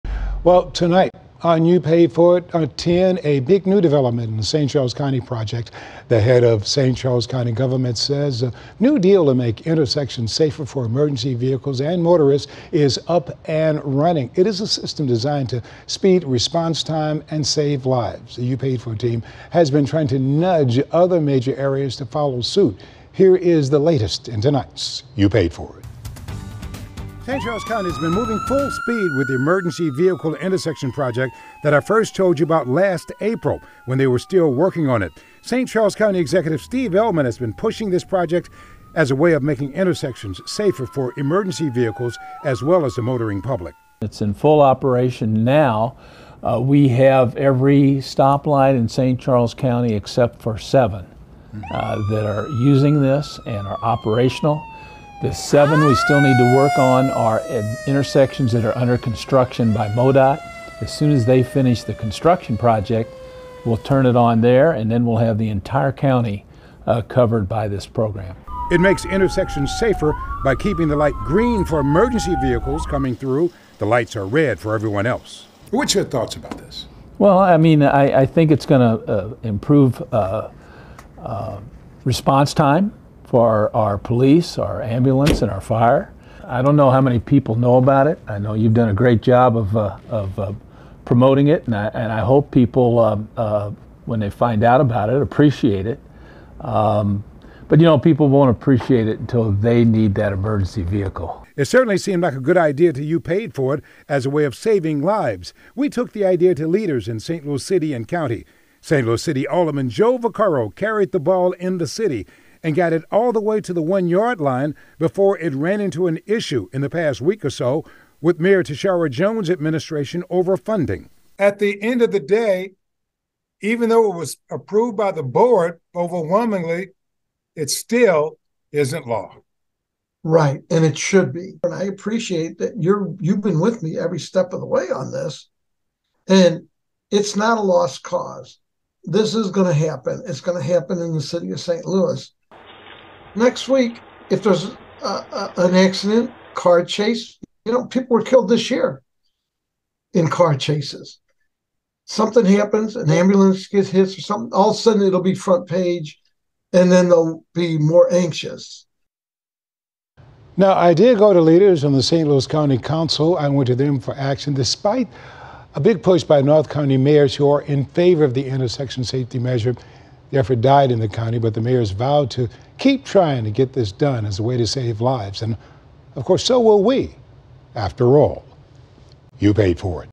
St. Charles County nearly finished implementing new traffic signal system for emergency vehicles
St. Charles County says its program to make intersections safer has been activated at almost all the crossroads.